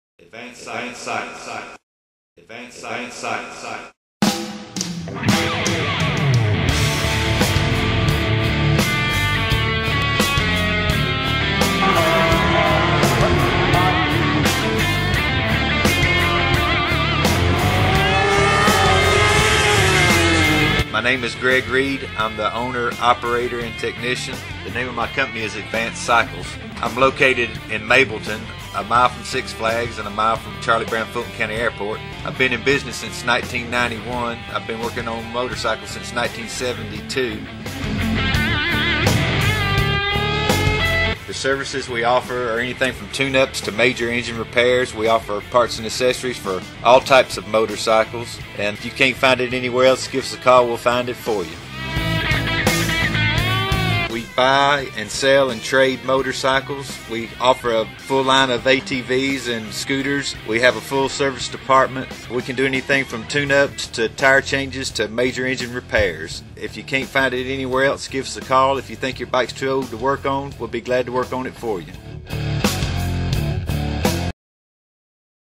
Advanced Cycles of Mableton, GA
Advanced Cycles provides parts and services from A to Z for Motorcycles, ATVs and Watercrafts since 1991.  Located at 6940 Mableton Parkway in Mableton, GA.    YouTube Video produced by BIE Media Group.